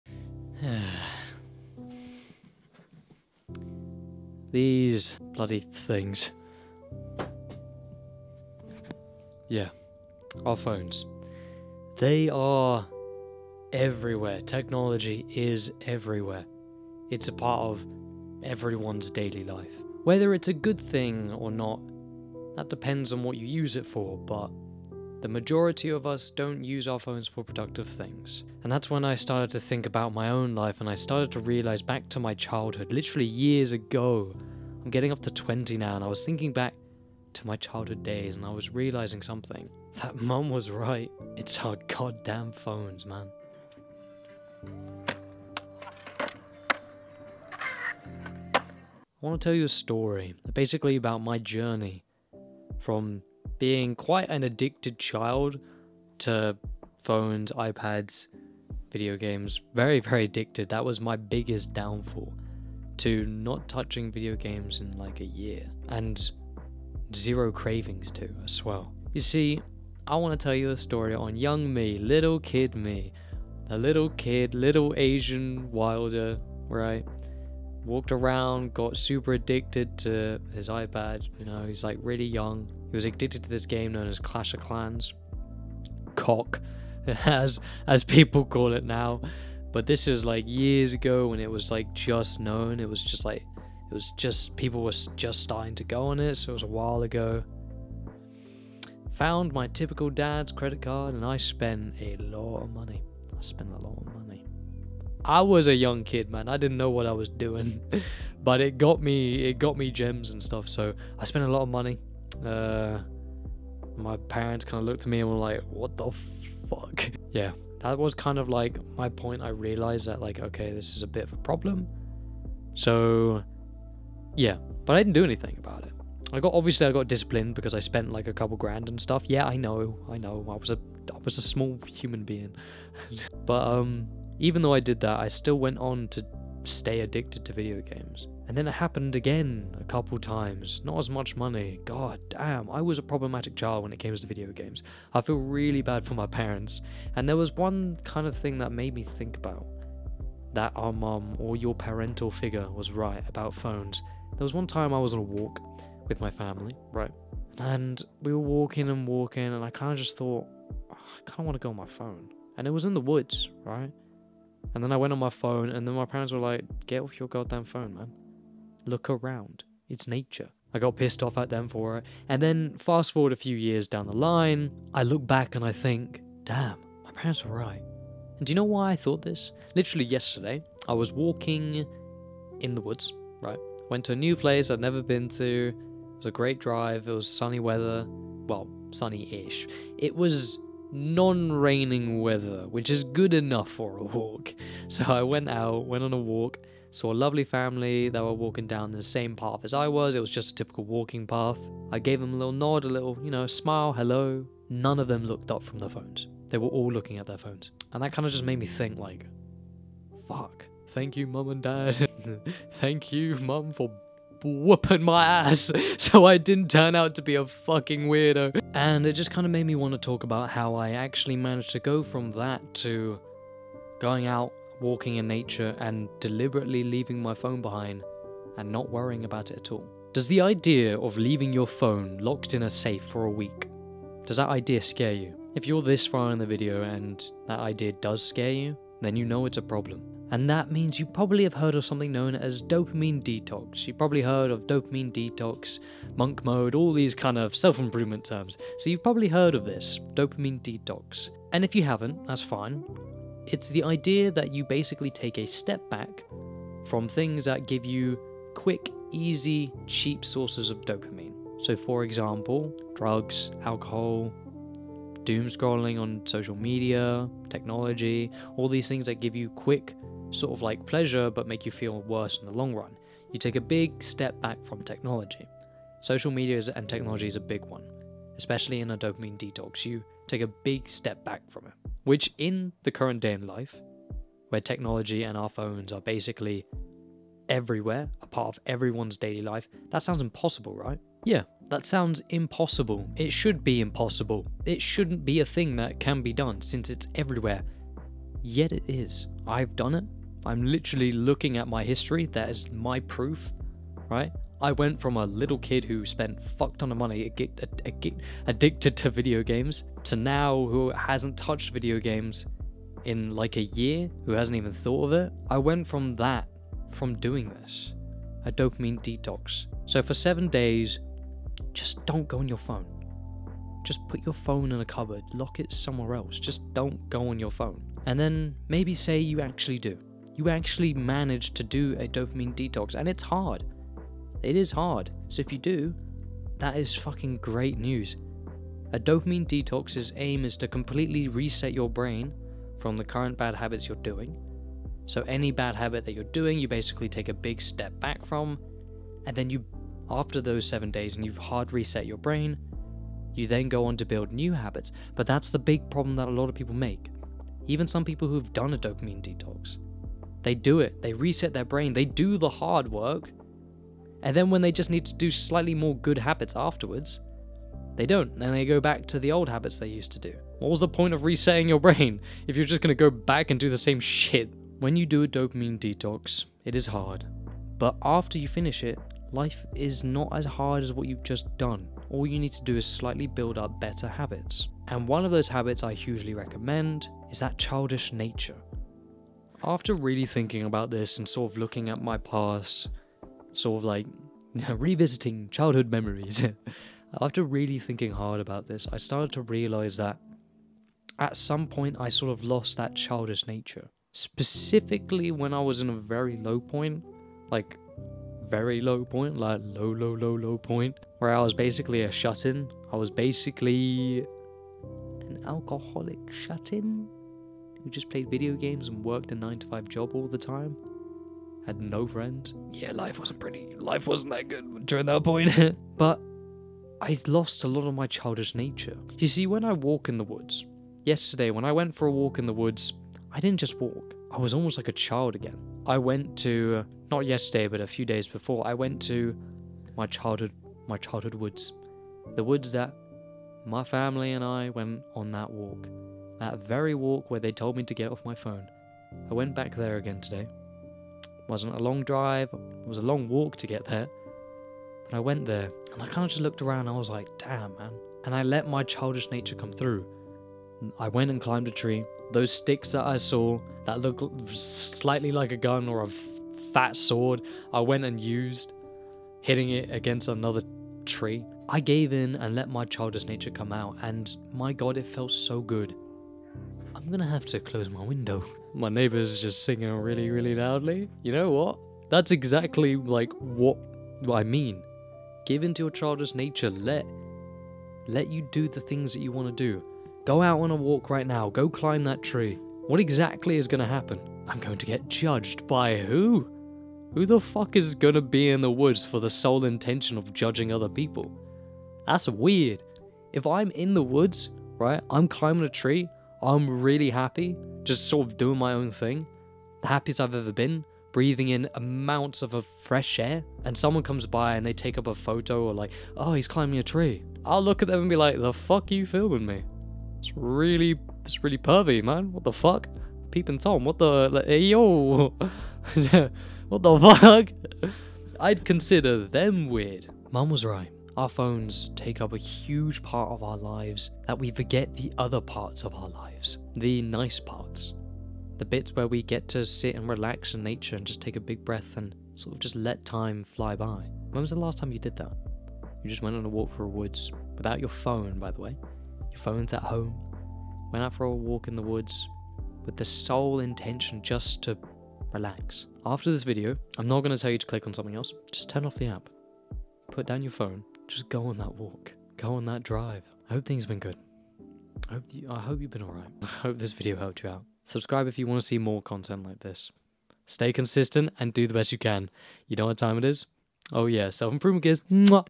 𝗪𝗵𝗲𝗻 𝘄𝗲 𝘄𝗲𝗿𝗲 𝗸𝗶𝗱𝘀.
Mum was right, it's our damm phones...

When was the last time you went out to walk in nature and just leave your phone behind?
If the idea of locking up your phone for a week out of your reach scares you then you need this...

A Dopamine Detox...